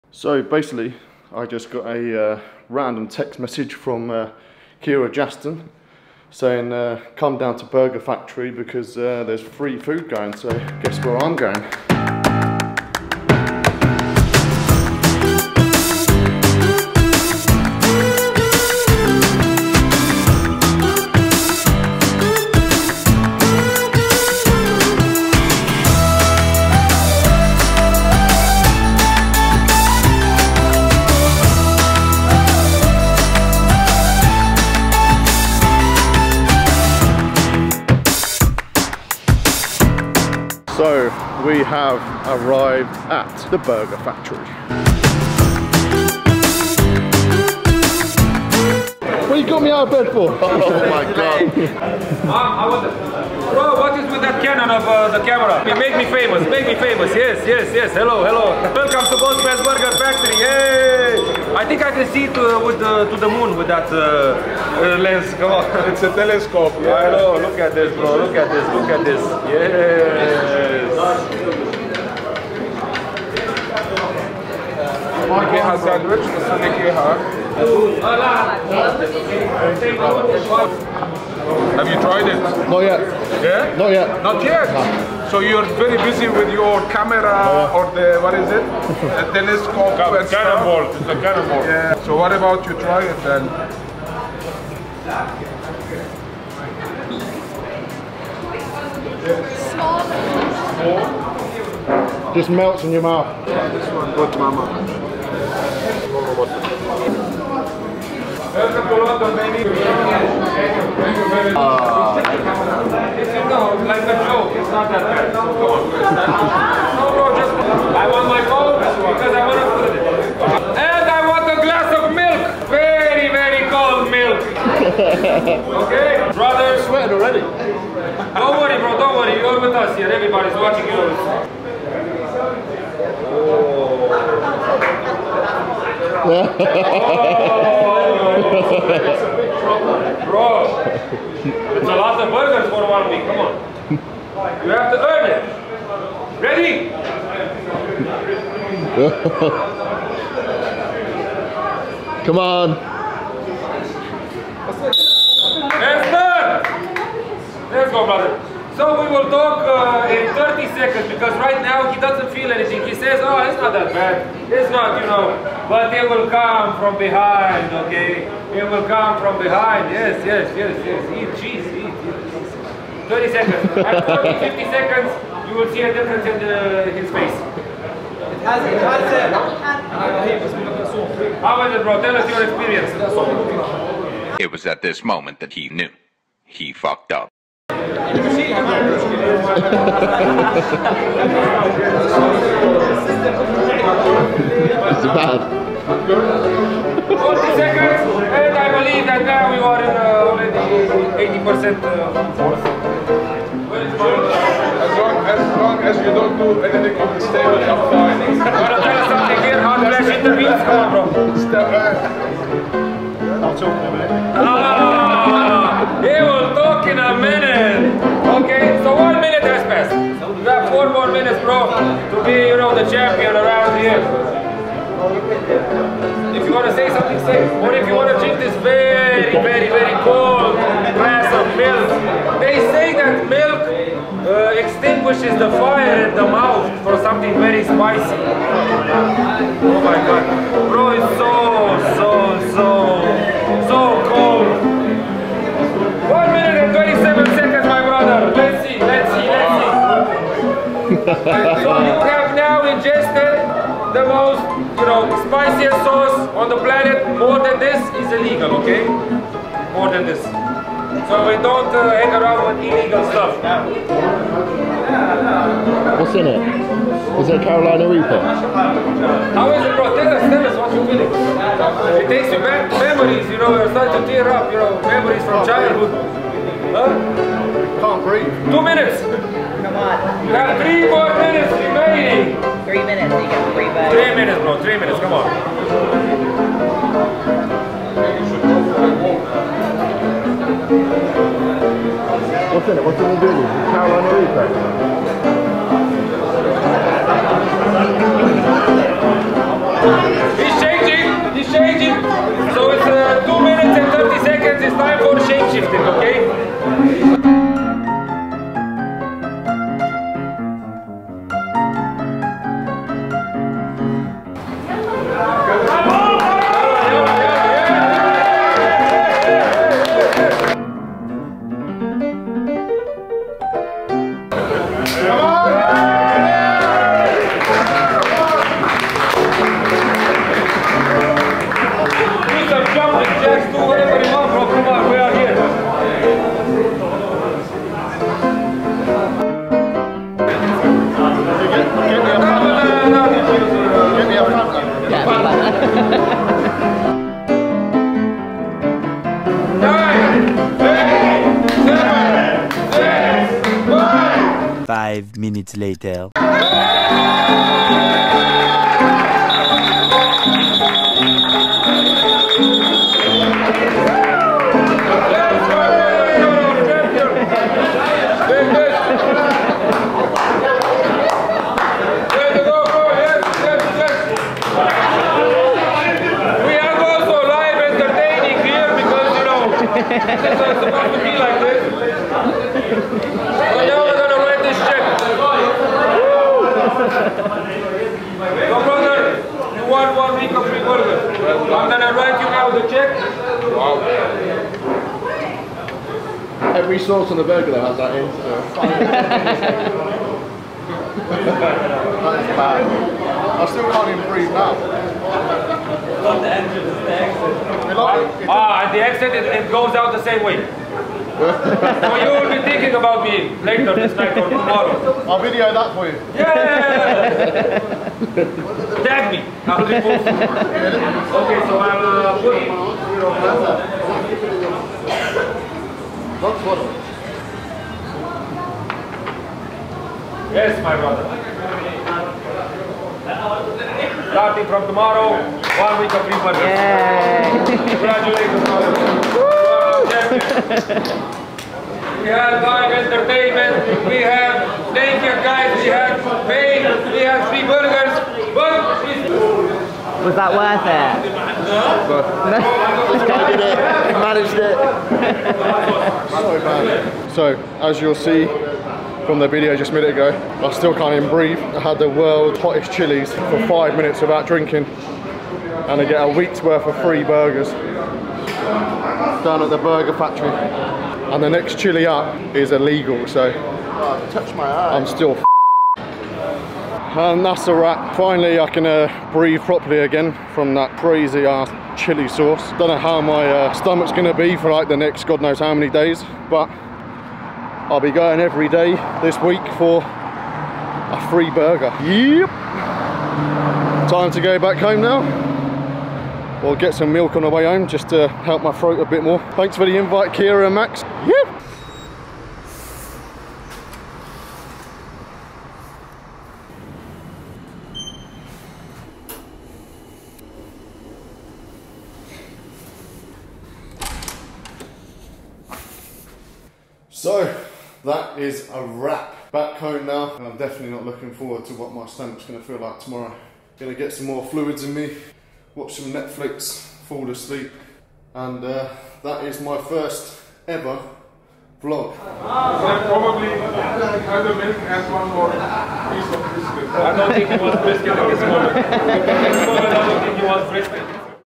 HOTTEST CHILLI SAUCE IN THE WORLD CHALLENGE IN DUBAI
Worlds hottest chilli sauce challenge at the @bossmansburgrfactory the cheat day headquarters in Dubai.

Eat some of the worlds hottest chilli sauce on some fresh cooked meat and not drink for 5 mins after to win a weeks worth of free burgers!

Location instagram: @bossmanburgrfactory